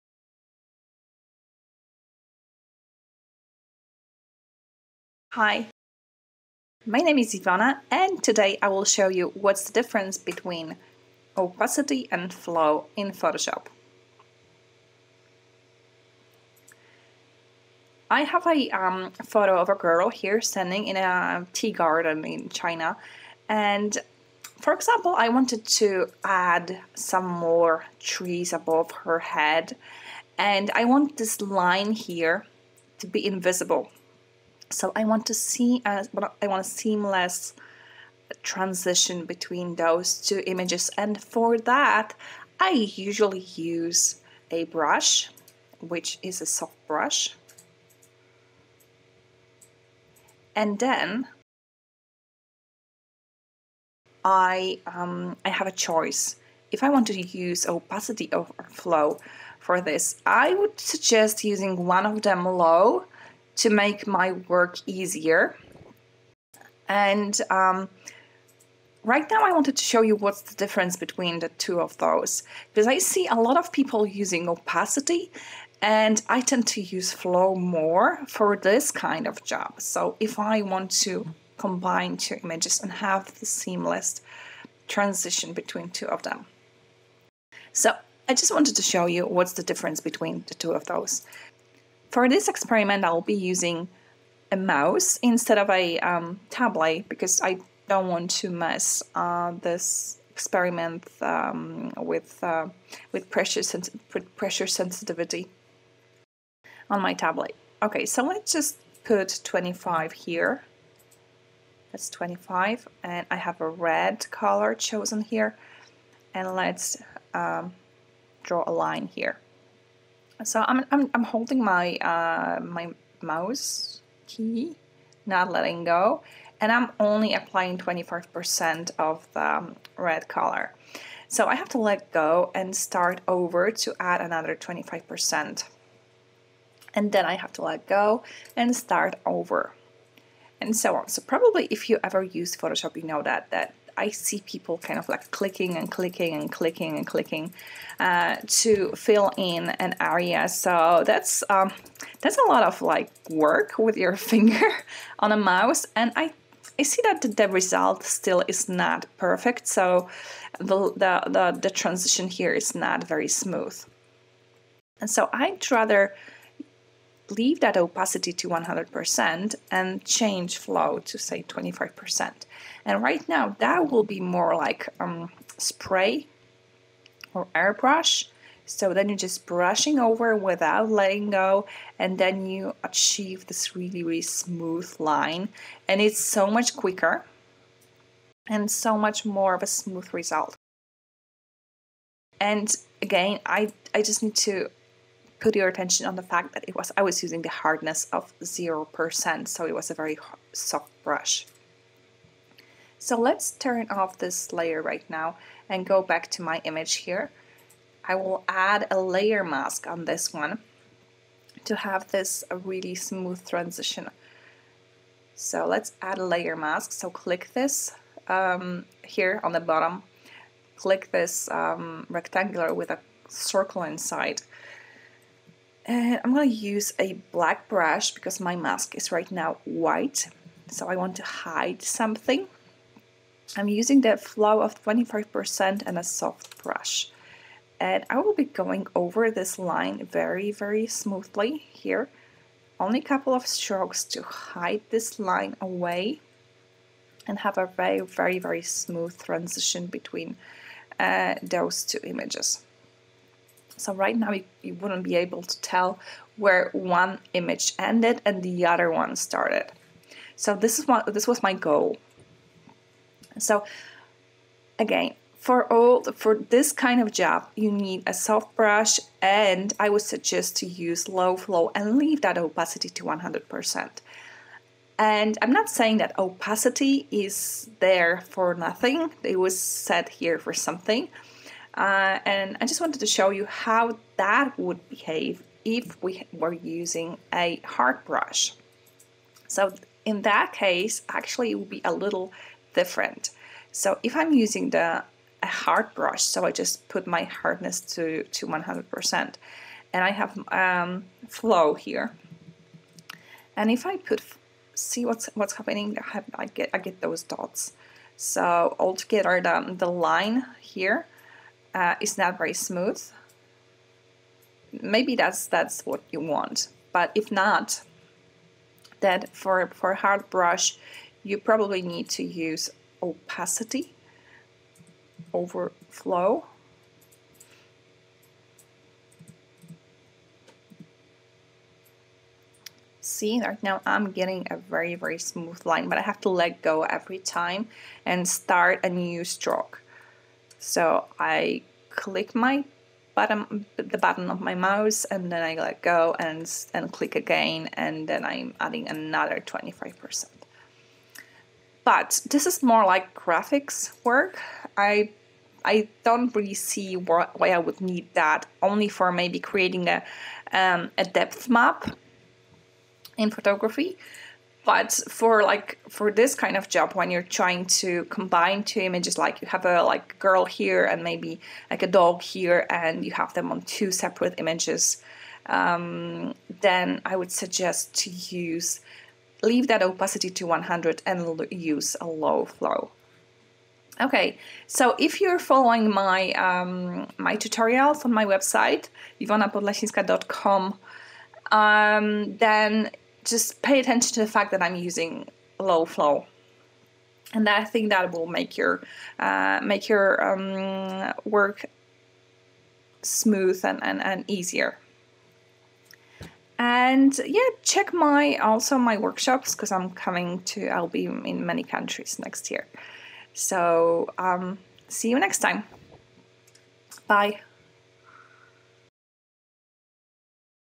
Flow vs opacity - which one I use most. Learn what's the difference
What's the difference between flow and opacity when using brushes in Photoshop. Learn which one to use to been two images together.